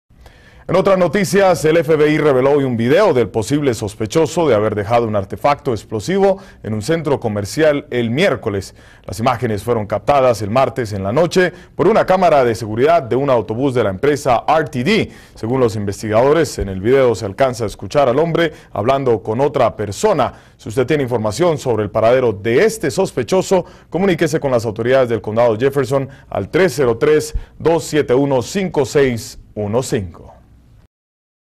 FBI Circula Video De Sospechoso
El FBI reveló hoy un video del posible sospechoso de haber dejado un artefacto explosivo en un centro comercial el miércoles.